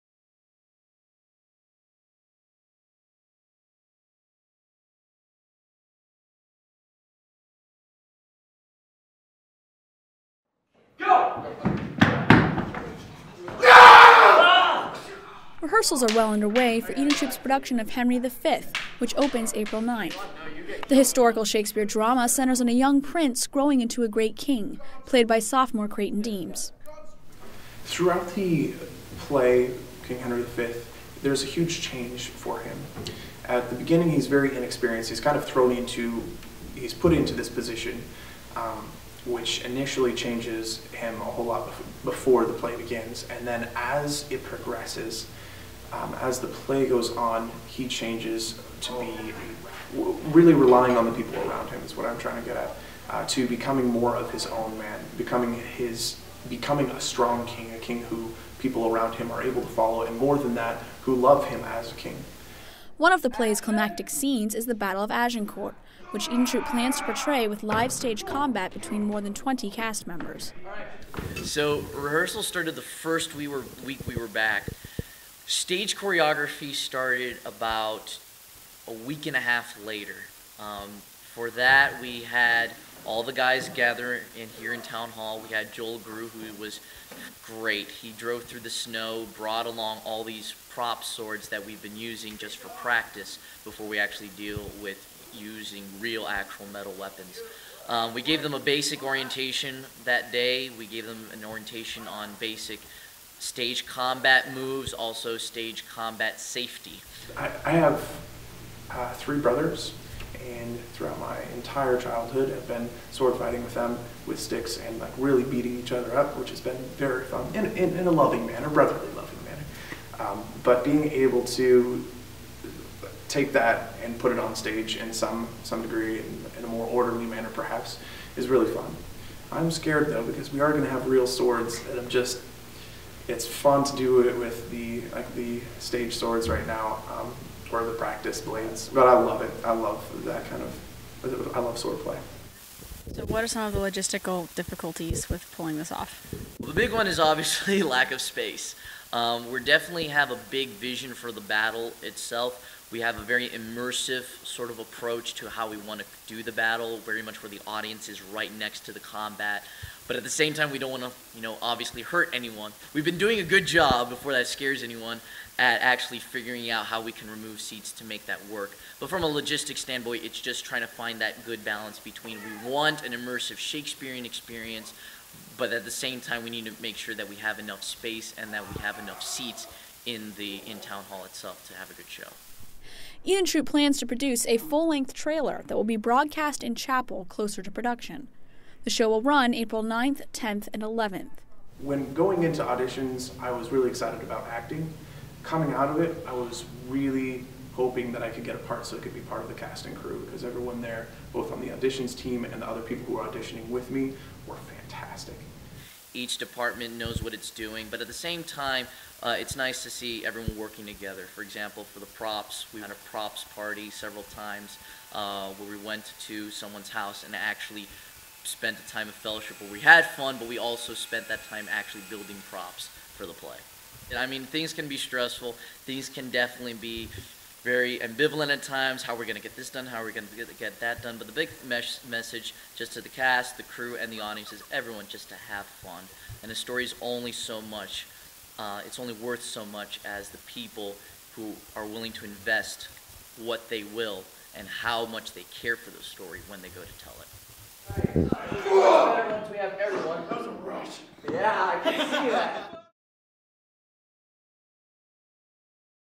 Henry V - Behind the Scenes
Watch as Eden Troupe practices its live-action battle scenes for its performance of Shakespeare's Henry V. Sophomore Creighton Deems (King Henry V) and junior Christian Fernandez (director) share the many details that go into making Shakespeare come alive at Patrick Henry College.

*Note - the 'real' swords referred to are real metal swords made for theatrical performances. They are not for actual combat.